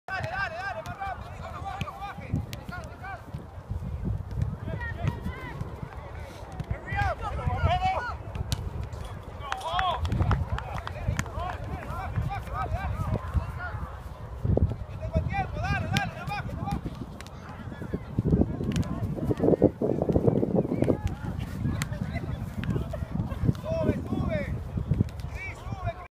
NJSC Ball Control Training
ball control training video under supervision of Coach Henry Perales.